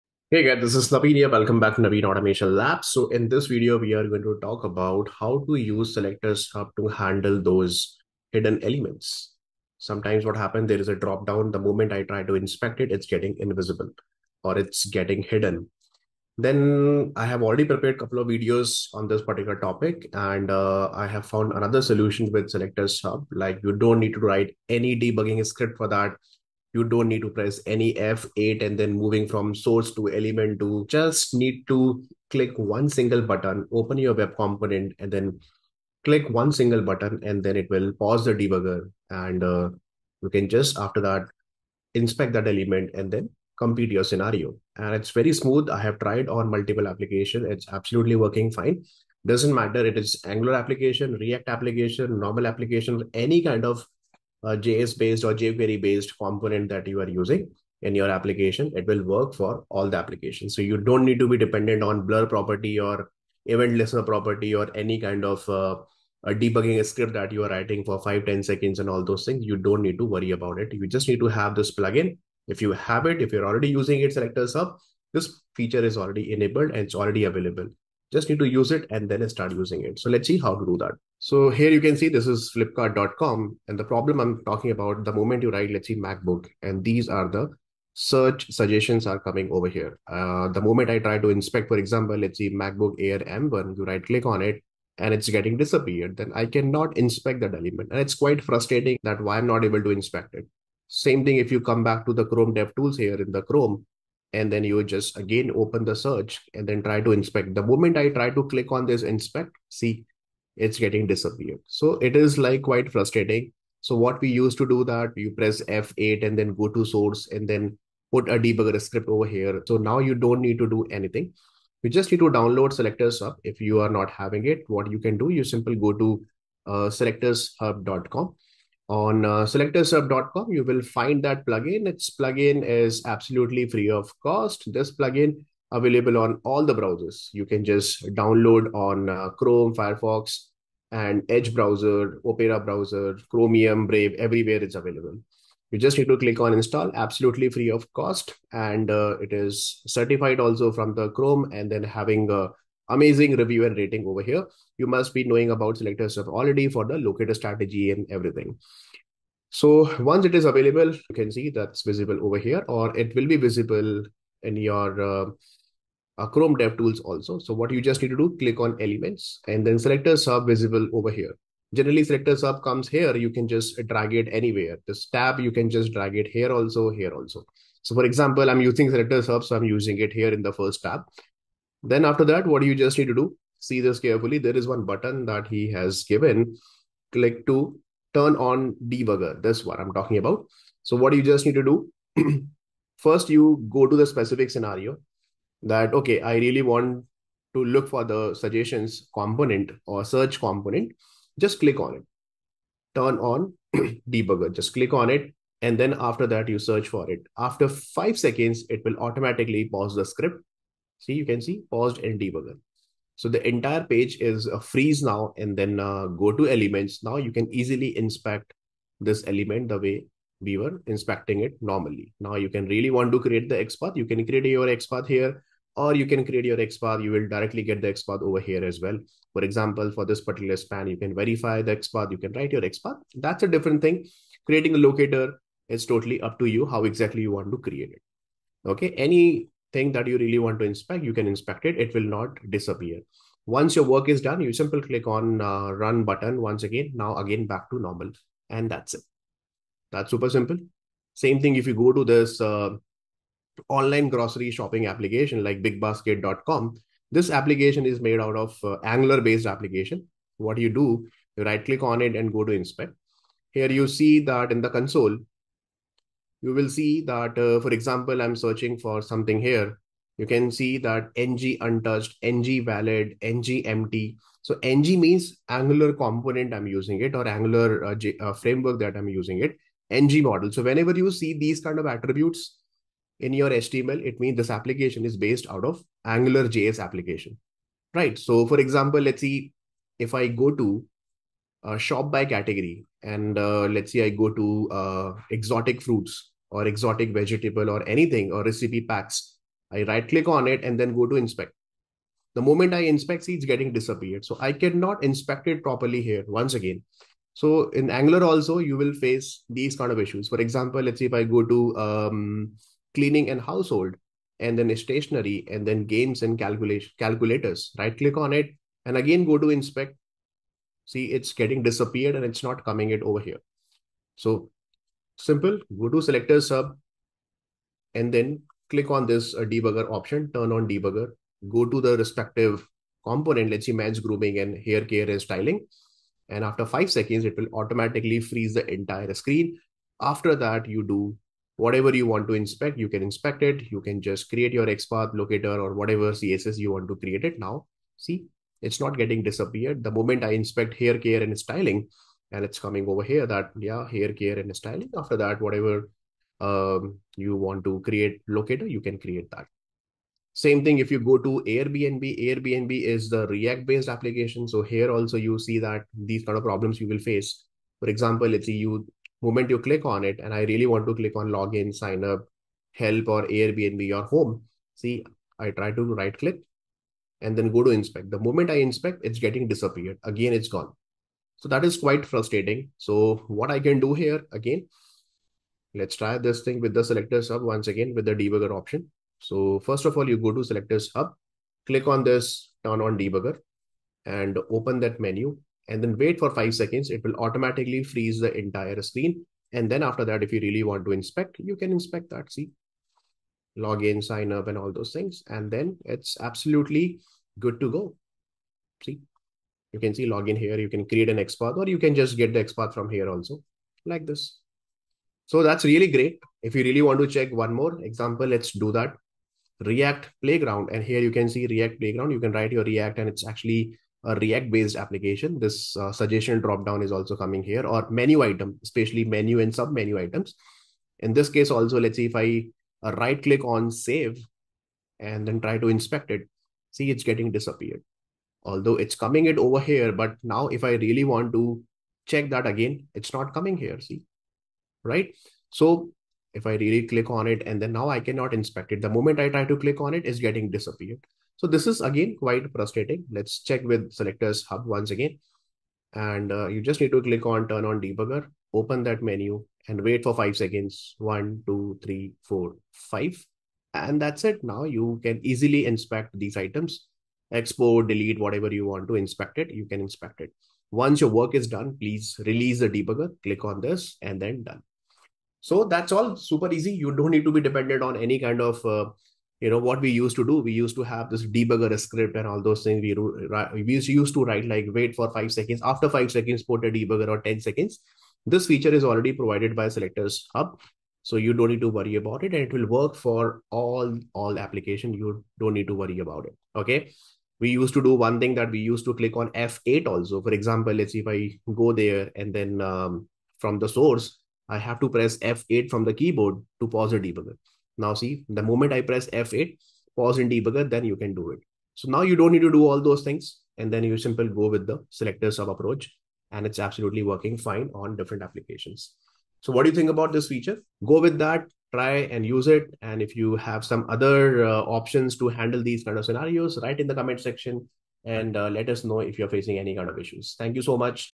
How To Inspect Hidden / Disappeared Elements In Just One Click
In this video, I have explained how to inspect hidden or disappeared elements using SelectorsHUB Debugger feature in Just One Click.

Naveen AutomationLabs Paid Courses:
GIT Hub Course: